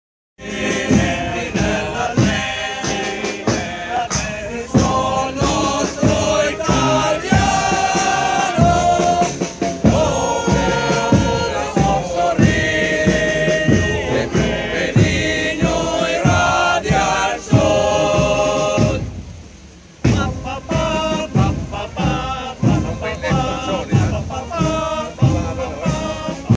Gli alpini sfilano a Perino per il Centanario dello scoppio della prima guerra mondiale  e per festeggiare il 65esimo anniversario di fondazione del gruppo